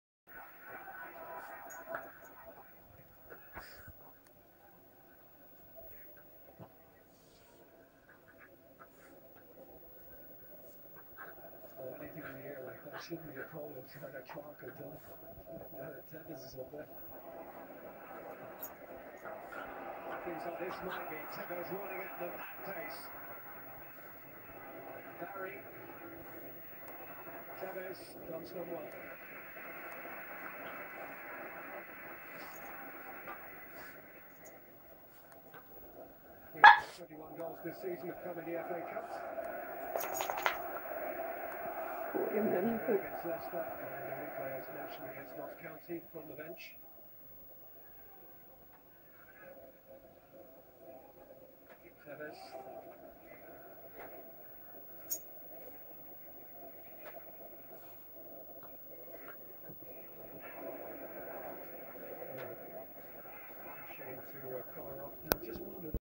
Introducing Bert & Bruno
Bert the border terrier playing with the newest member of the family, Bruno the bengal kitten.